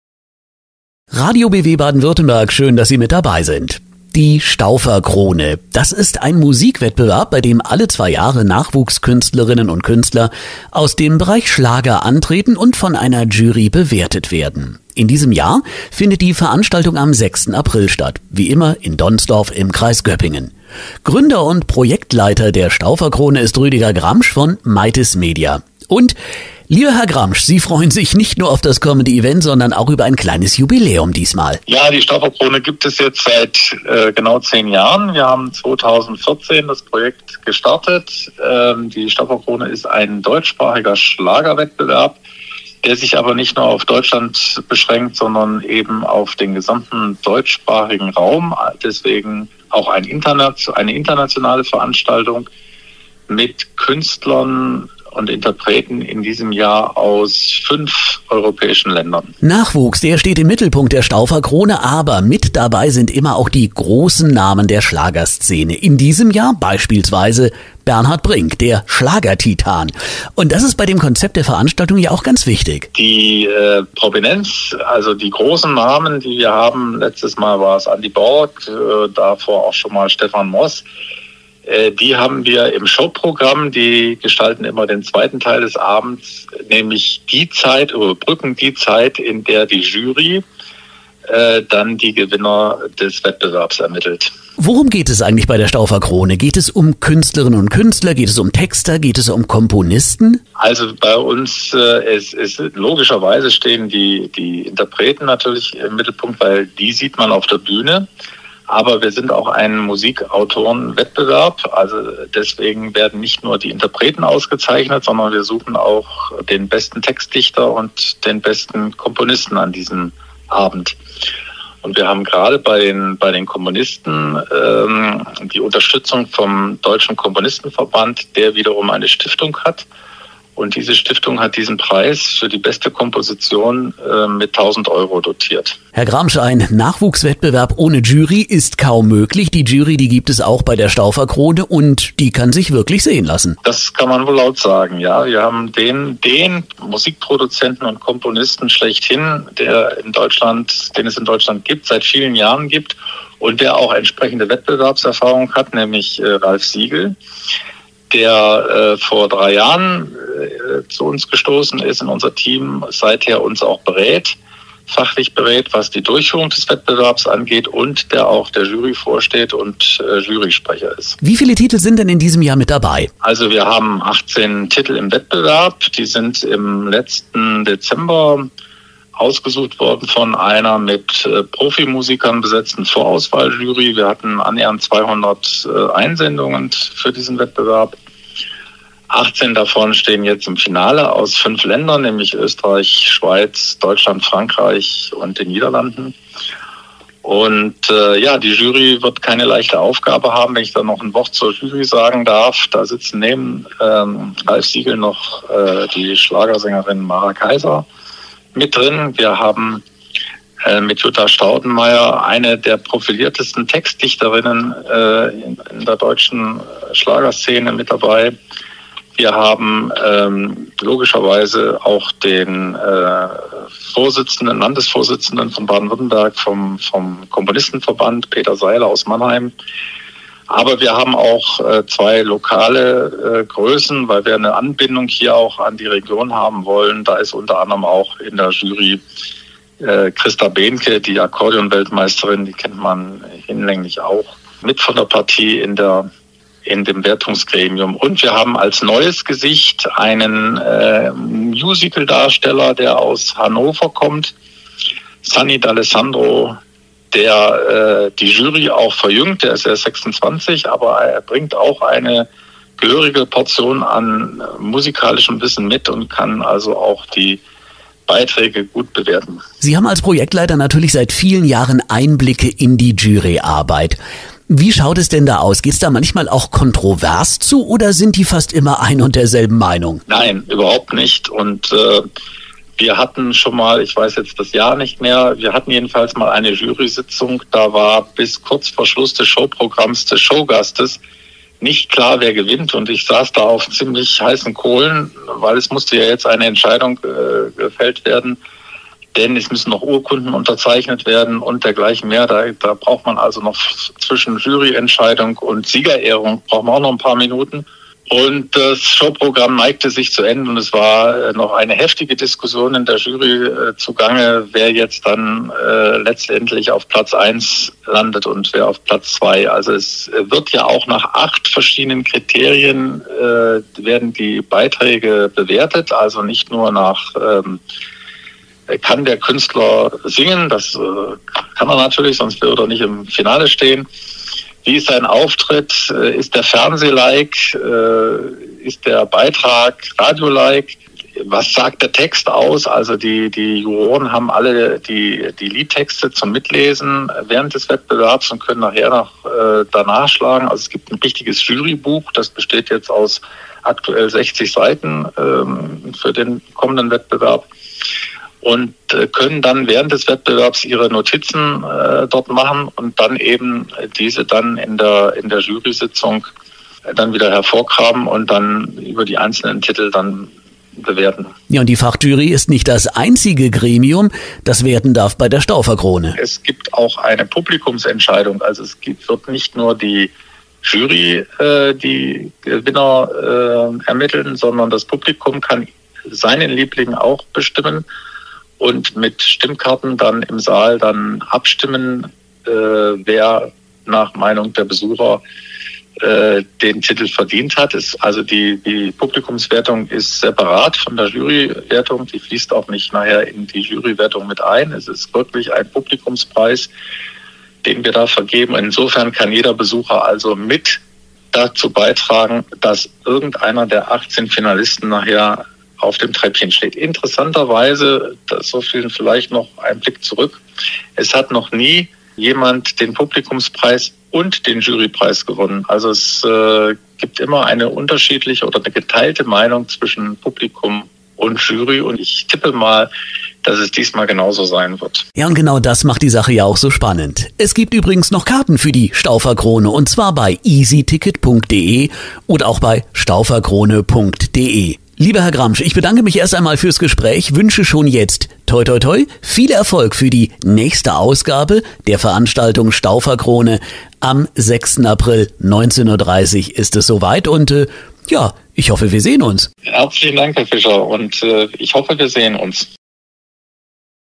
"Stauferkrone 2024" - Projektleiter Rüdiger Gramsch im Interview bei Radio BW Baden-Württemberg
Die "Stauferkrone" feiert 2024 ihr zehnjähriges Jubiläum. In diesem Jahr treten 18 Nachwuchskünstlerinnen und -künstler gegeneinander an und werden sowohl von einer Fachjury als auch dem Publikum in getrennten Abstimmungen bewertet. Projektleiter Rüdiger Gramsch von MaitisMedia über einen Schlagerwettbewerb, der seriös und fundiert Newcomer fördert.